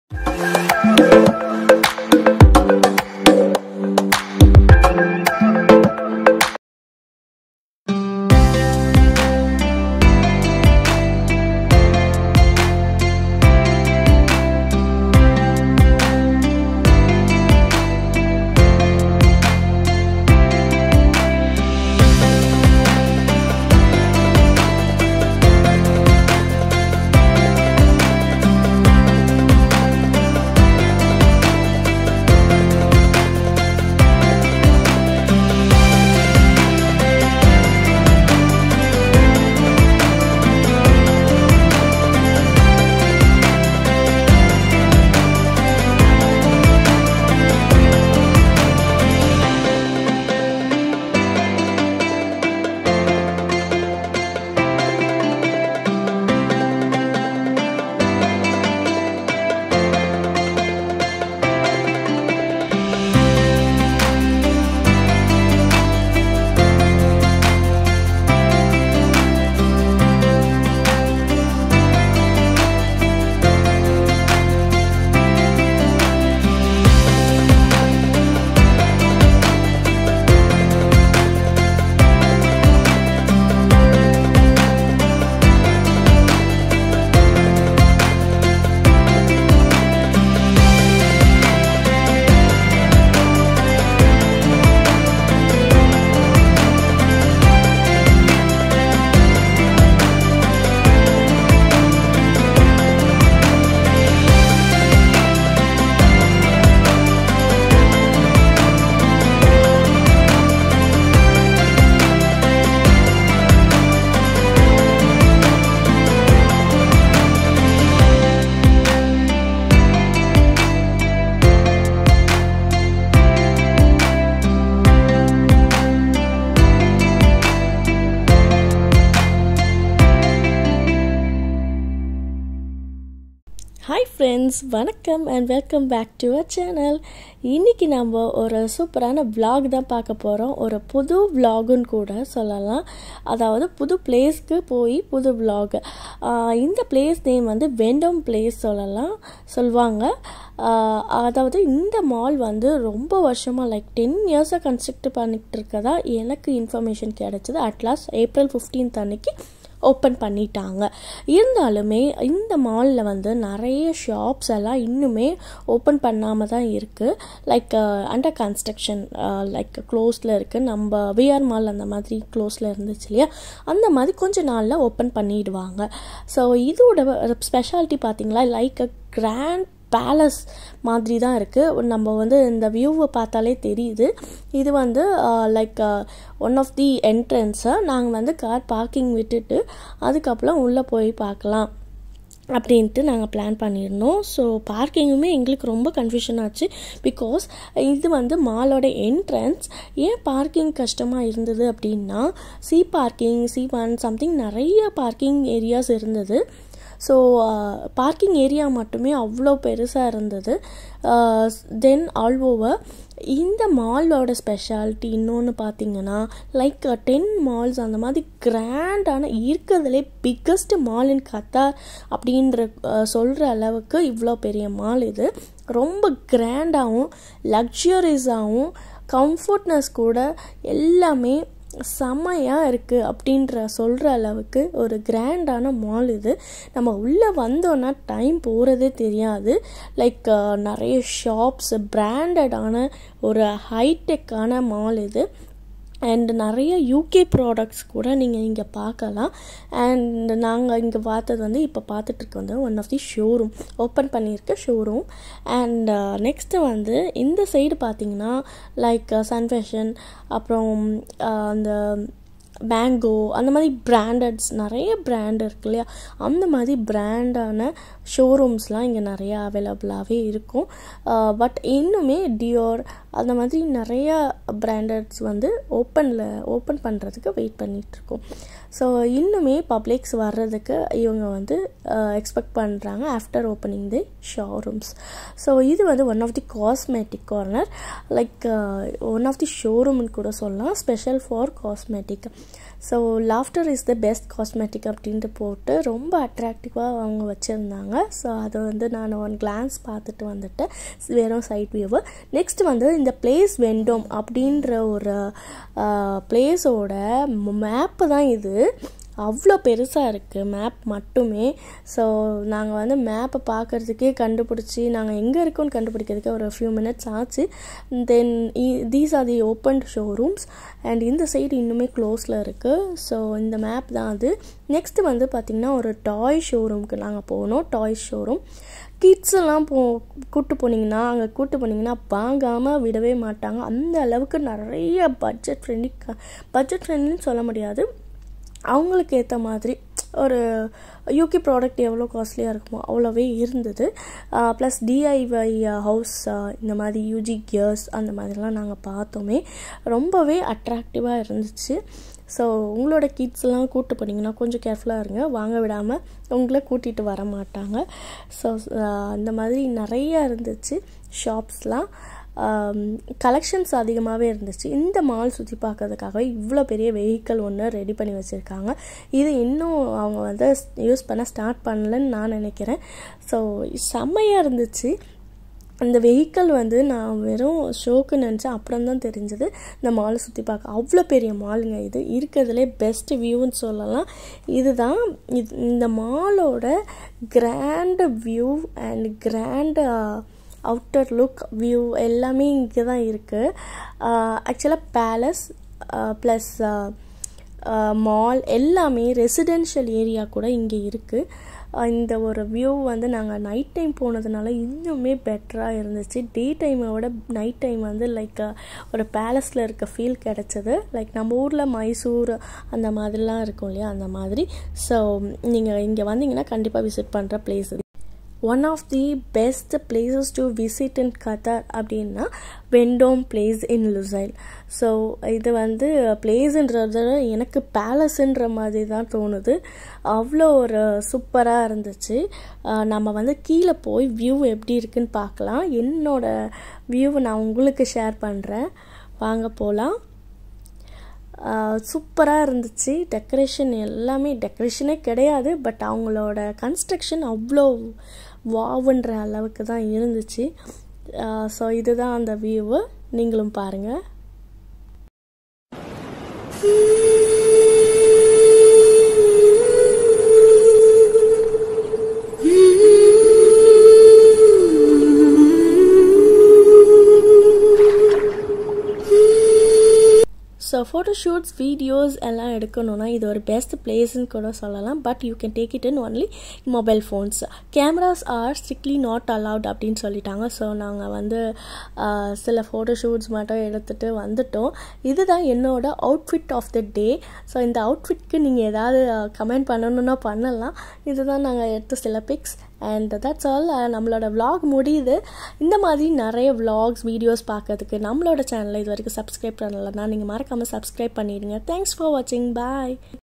NEW GRAND & MOST LUXURIOUS MALL IN QATAR😃😍🇶🇦🔥| VENDOME PLACE WATCH FULLY FOR MORE INTERESTING INFO'S
New Vendome place in Qatar🇶🇦 It was an amazing and one of the best place to visit in Qatar.. We enjoyed the whole view of the Mall, which is as similar & more luxurious as Palace..

Your likes👍🏻 are our motivation and your support is our Spirit🔥